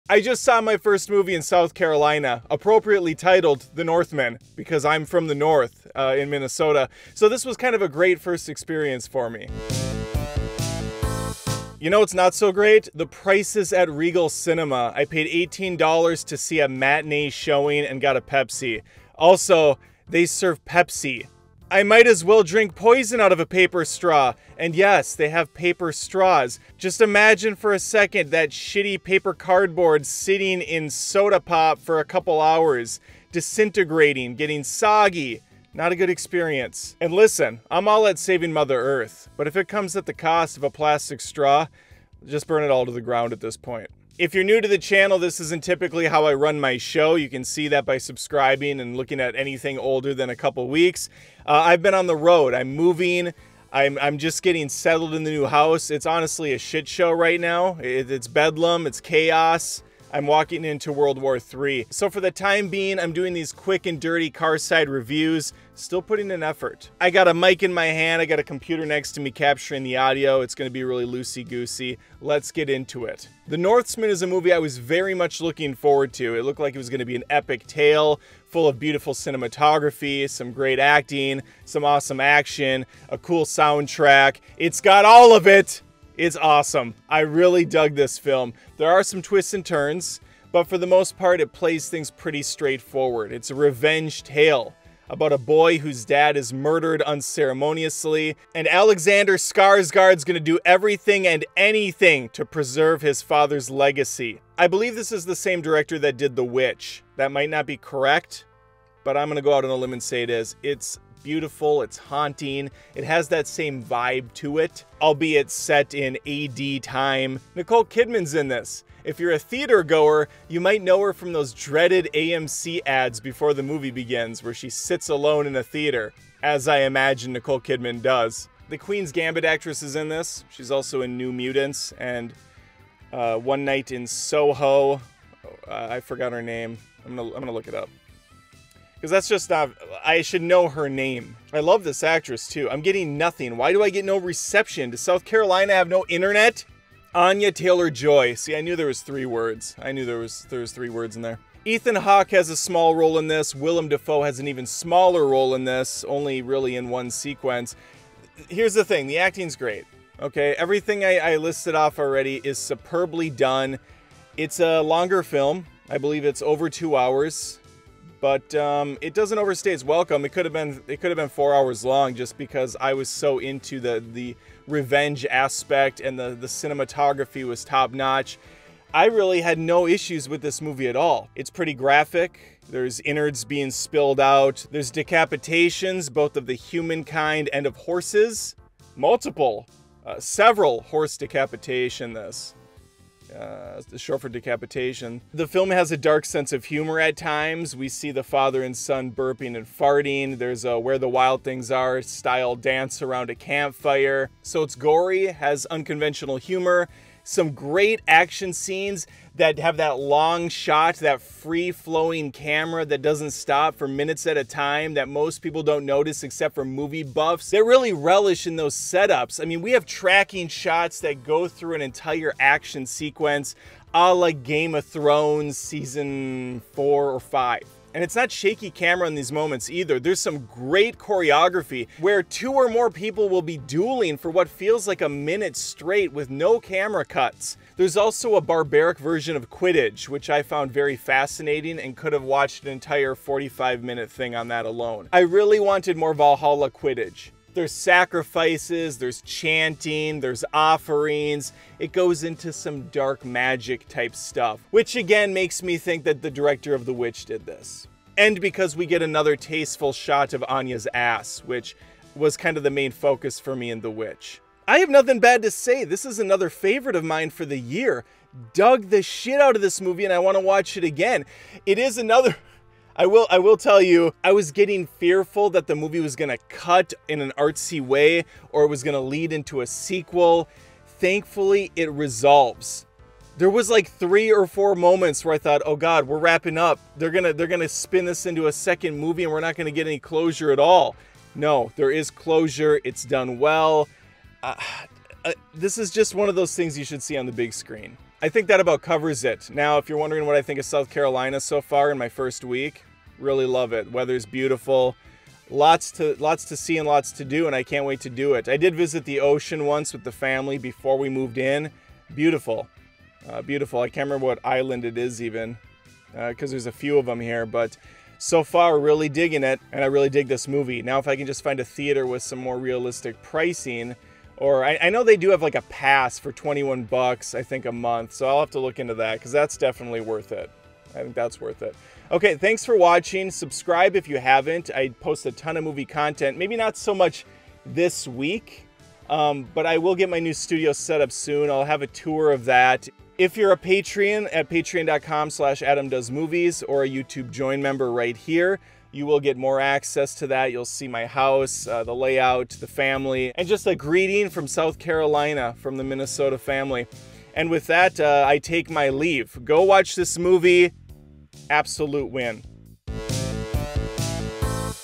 The Northman Movie Review
What a great month for cinema! The Northman was a film I was highly anticipating and it didn't disappoint. In fact, it blew me away! The beautiful cinematography, fantastic performances, killer score and brilliant tracking shots are just some of the many things to love about The Northman. See this film!